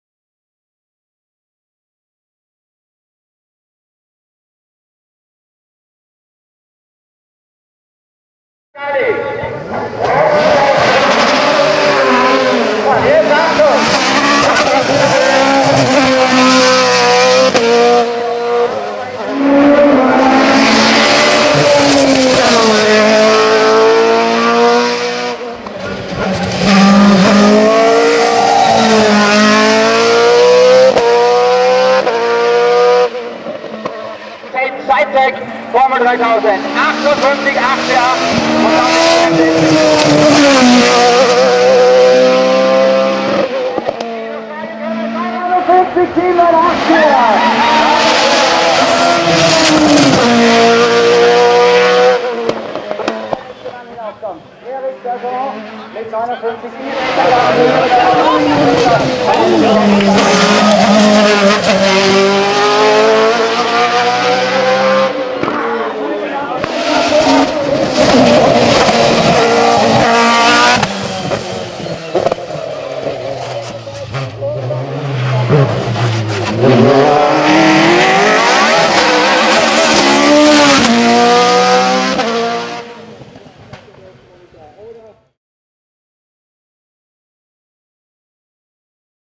Eric Berguerand/LOLA - Kitzeck 2013
Der Schweizer Eric Berguerand gewinnt nach einer Anreise von fast 900 km das Gedenkrennen in memoriam Hanspeter Laber mit neuem Rekord. Danke Eric, dass du dabei warst!